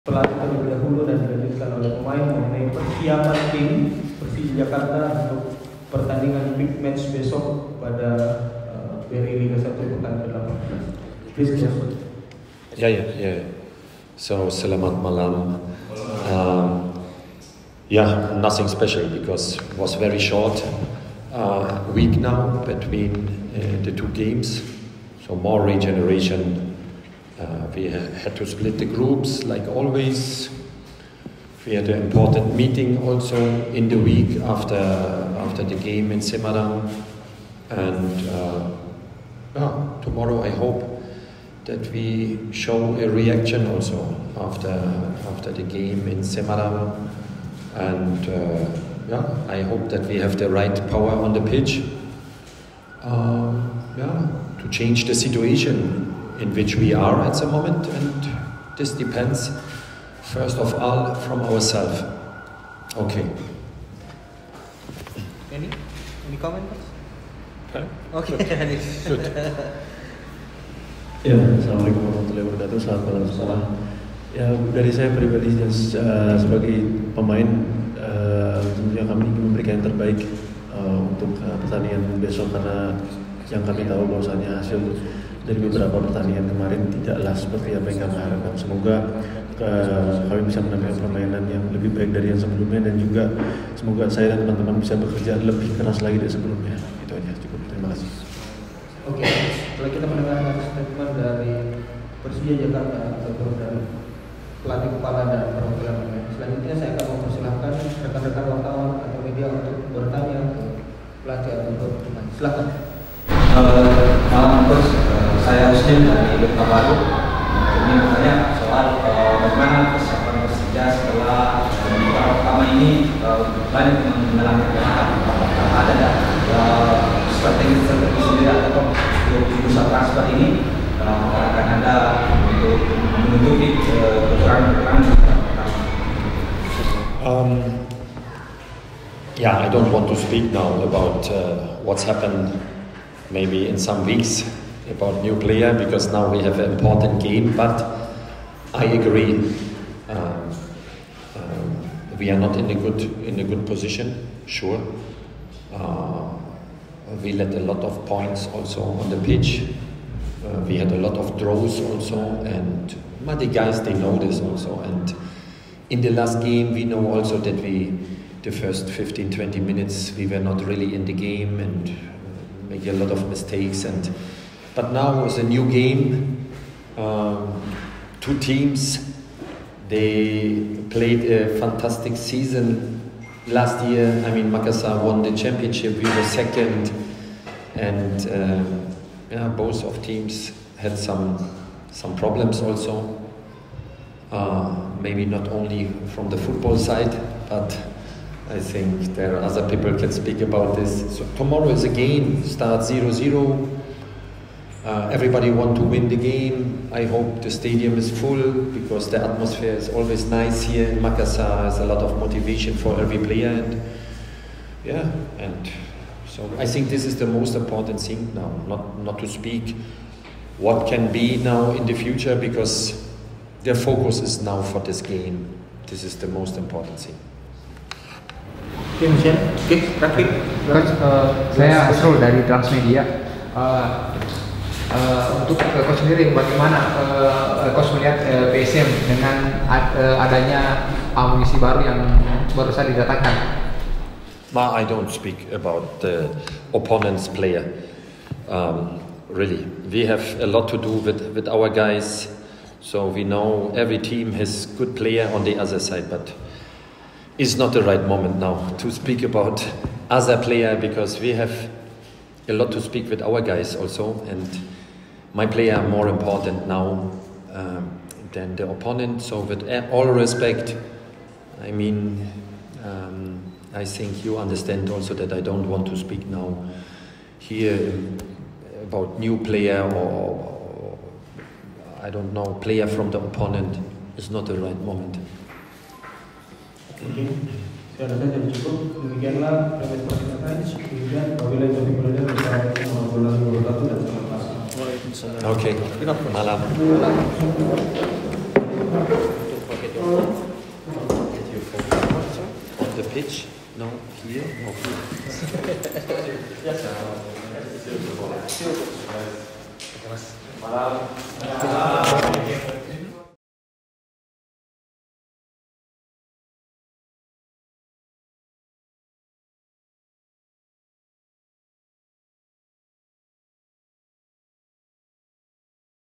Thomas Doll: Kami Berharap Mengeluarkan Penampilan Terbaik di Lapangan Melawan PSM Makassar
Persija akan bertamu melawan PSM pada laga perdana paruh musim kedua Liga 1 2023/2024. Laga tersebut akan tersaji di Stadion Gelora B.J. Habibie, Parepare, Jumat (3/11/2023), pukul 19.00 WIB.

Pelatih Persija, Thomas Doll mengatakan tim asuhannya lebih terfokus pada pemulihan fisik pemain dalam masa persiapan. Hal itu disebabkan Tim Ibu Kota baru saja bertandang melawan PSIS pada pekan lalu (29/10/2023).